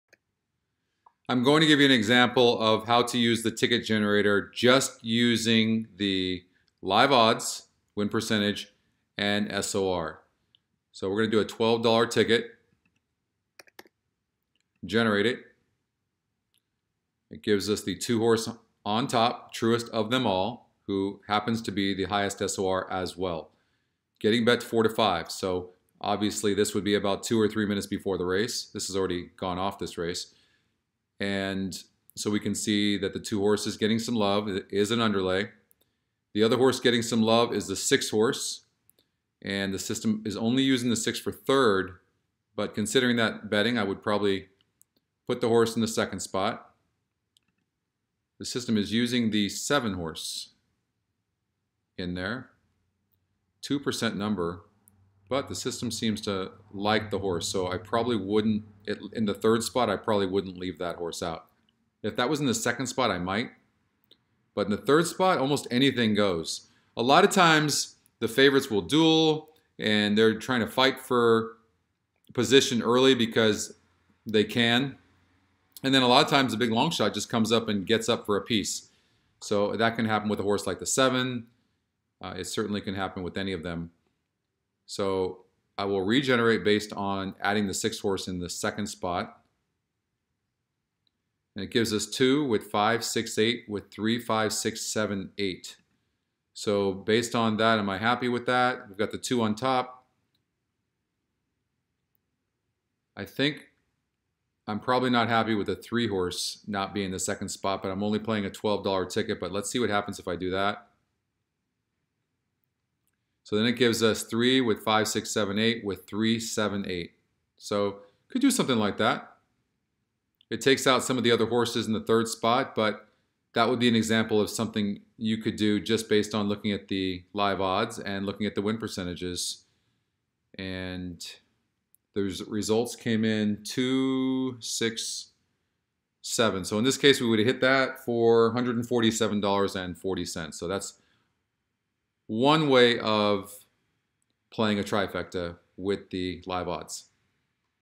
Using Live Odds with the Ticket Generator
In this video Scotty demonstrates how to utilize the live odds feature with the Ticket Generator.

Let artificial intelligence give you an edge with EquinEdge.